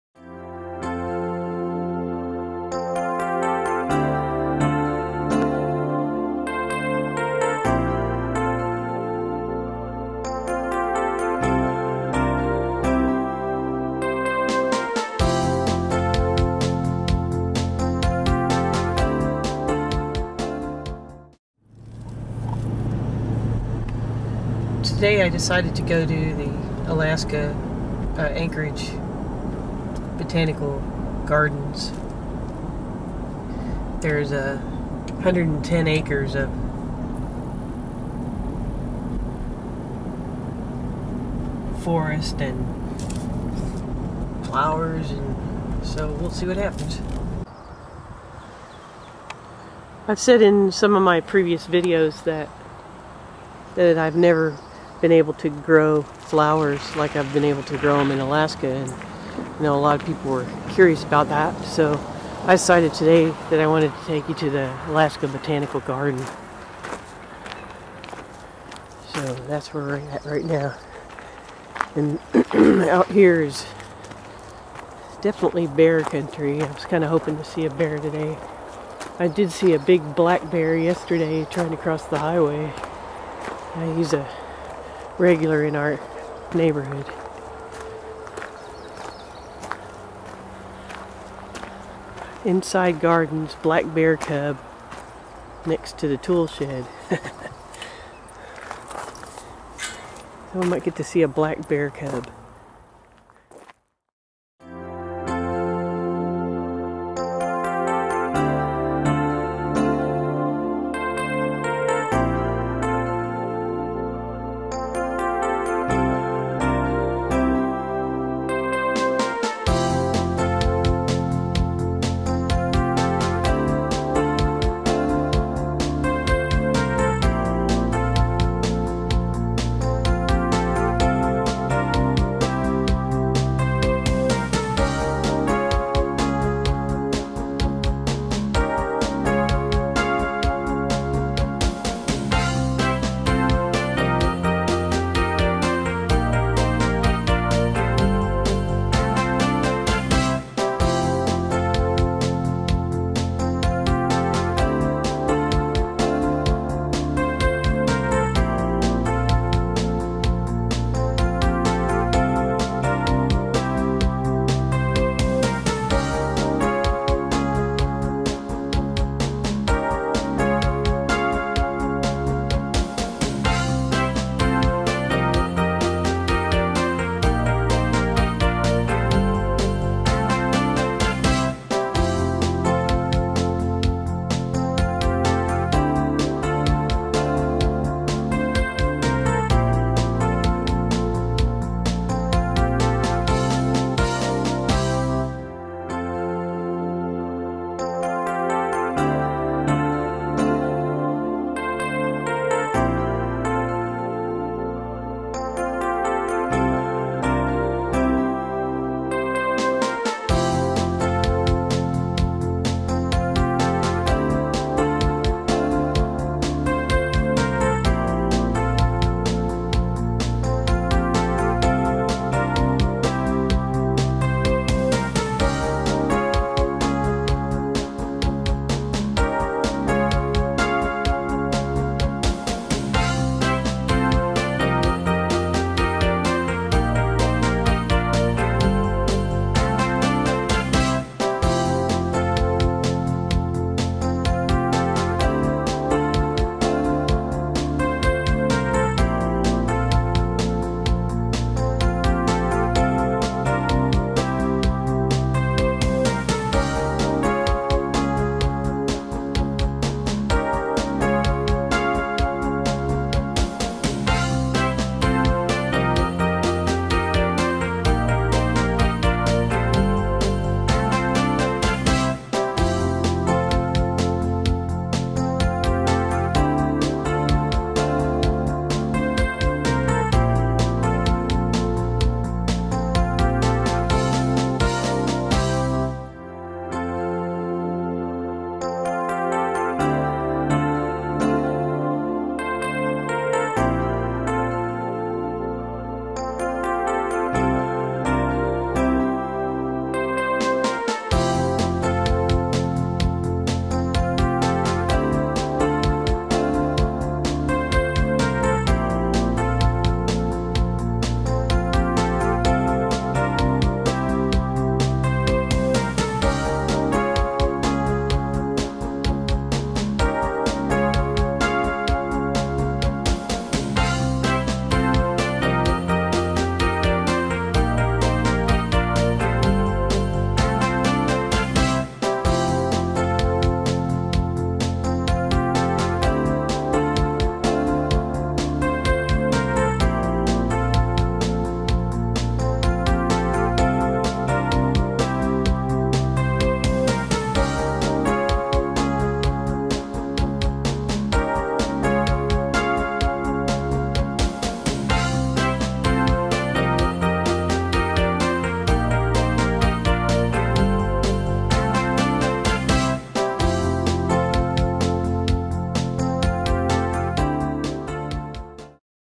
Episode 95: What The Heck Grows In Alaska? Part One
I take you to a beautiful place to show you something that might surprise you.